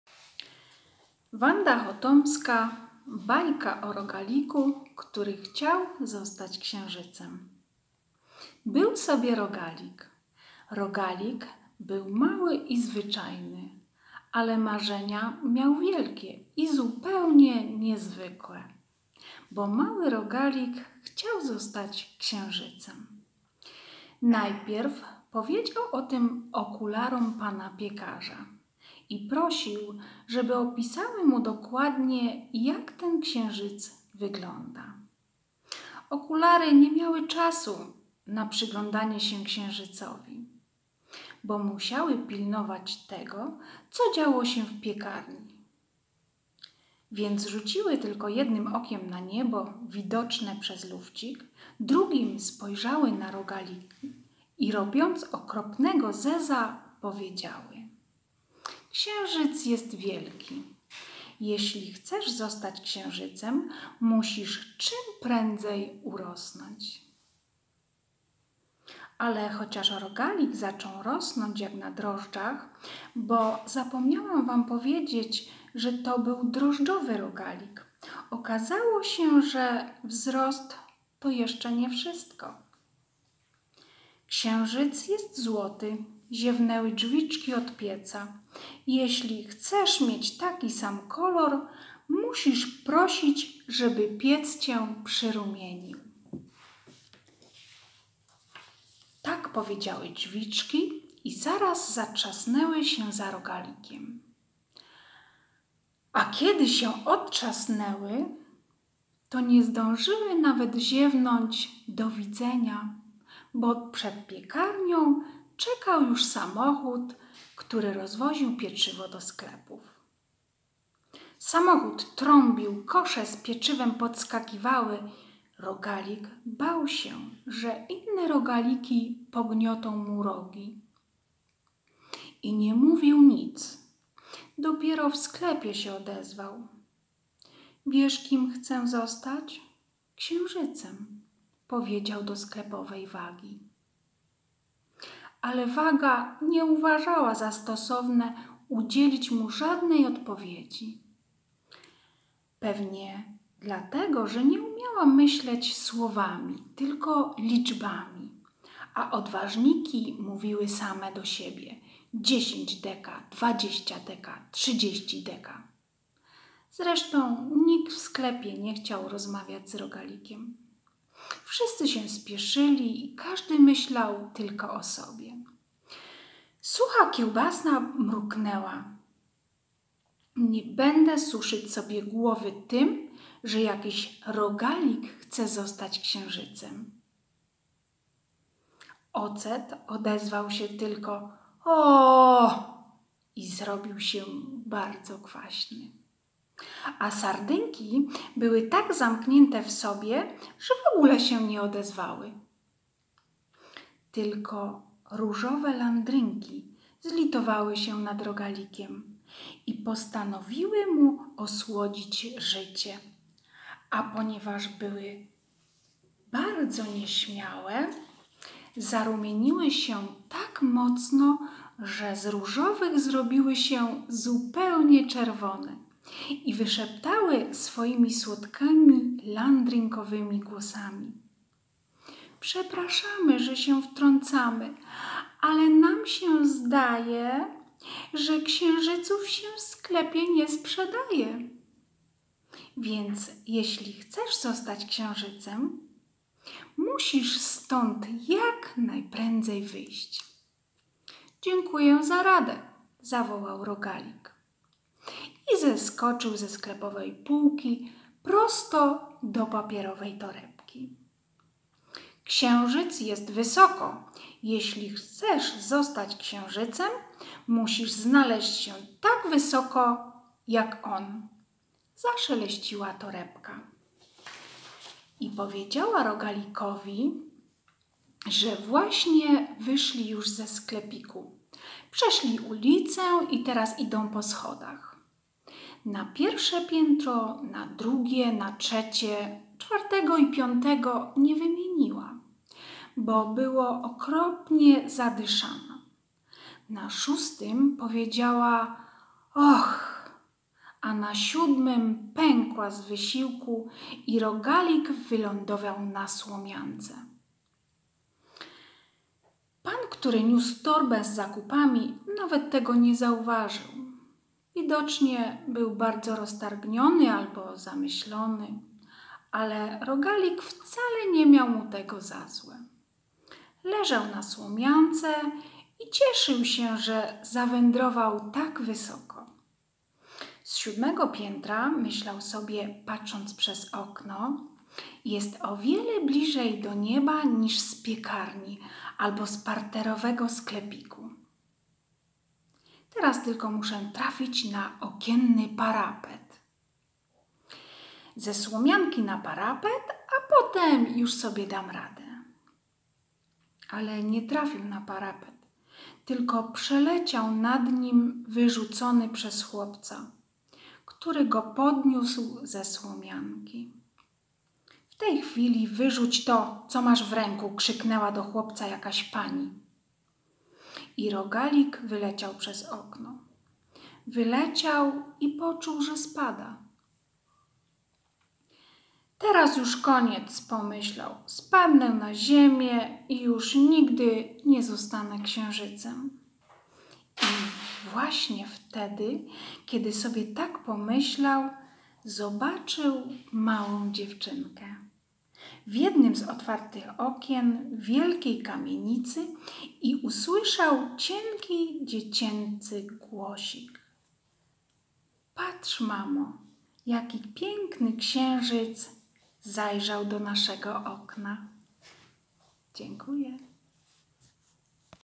"O rogaliku, który chciał zostać księżycem" Wandy Chotomskiej, czyta Jola Rogala.
ŁĄCZYTKA cz. 13